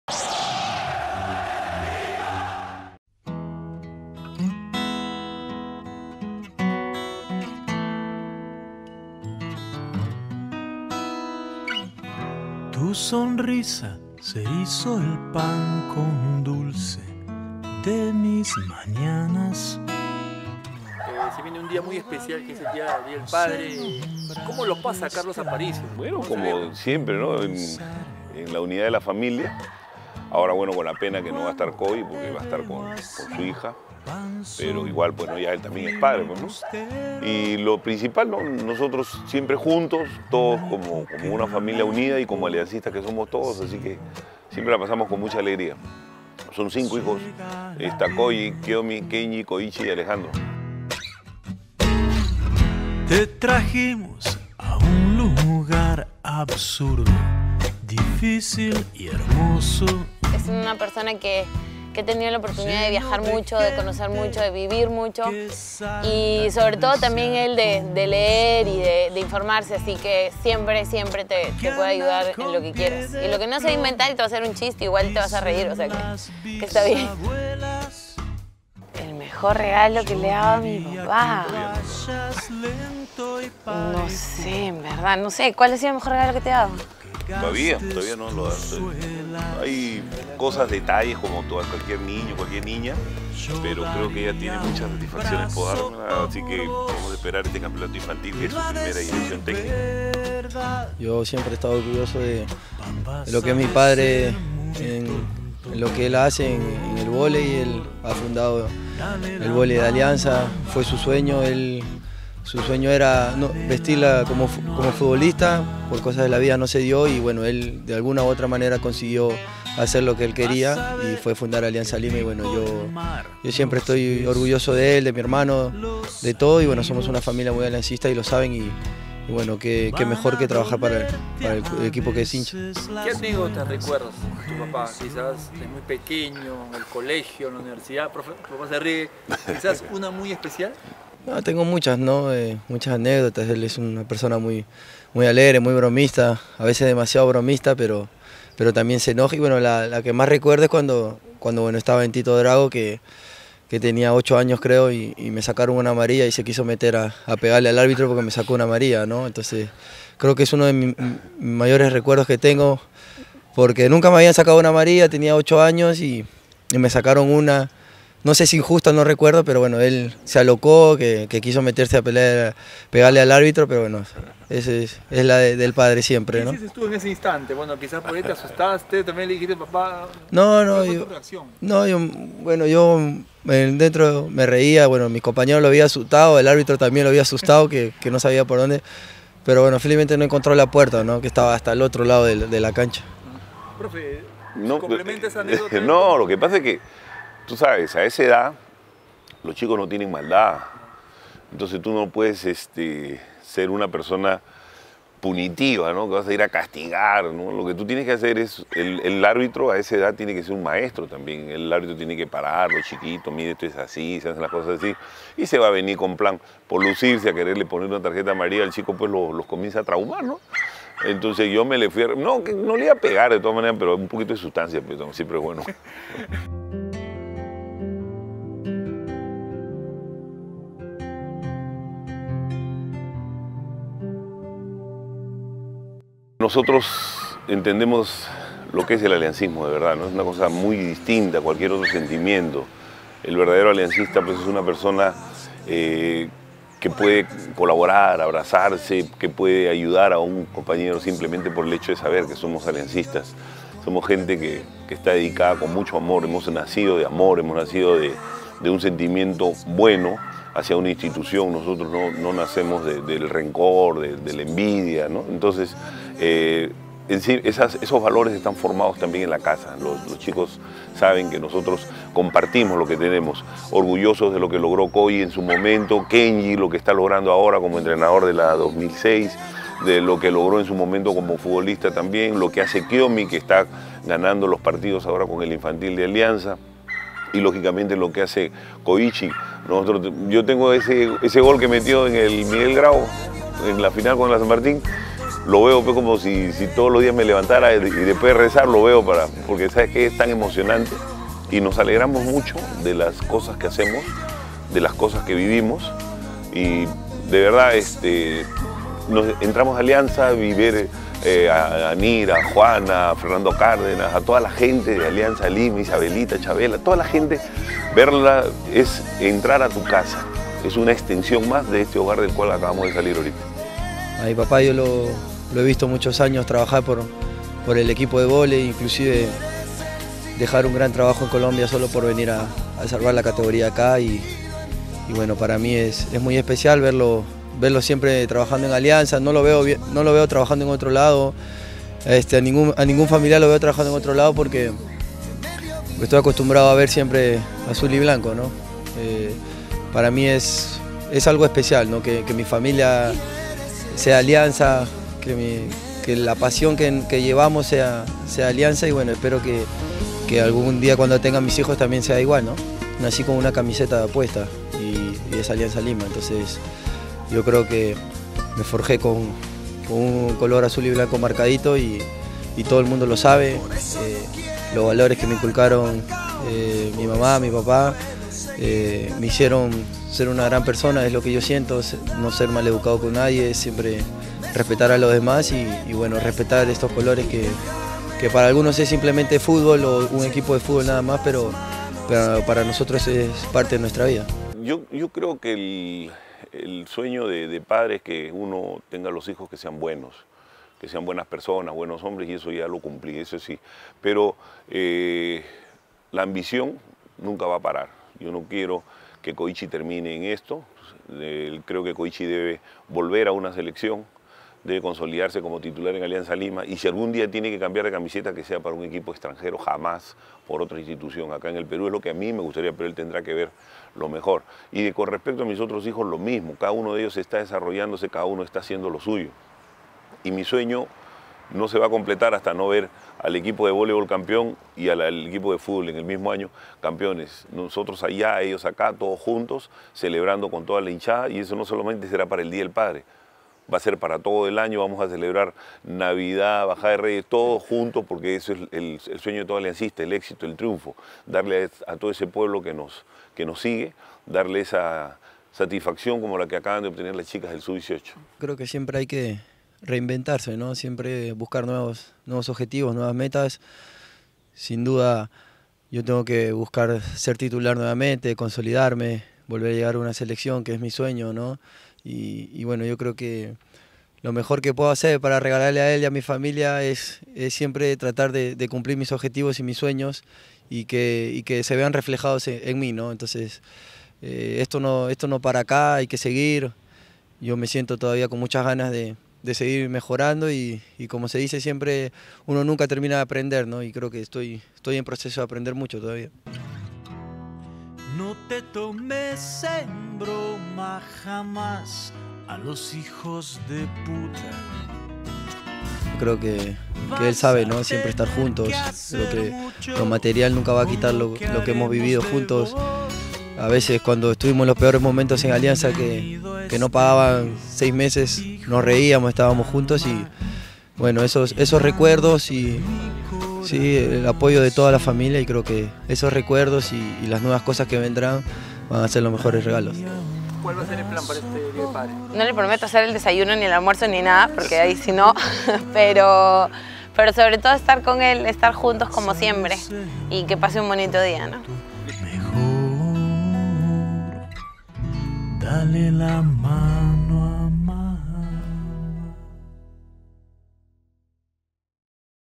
¡ Carlos Aparicio, y las razones de su vida !
El entrenador de Vóley, Carlos Aparicio, resaltó que  el “Día del Padre”, es muy  especial, porque siempre lo celebra en compañía de sus hijos. Además detalló lo que significan en su vida,  Koichi y Kyomi.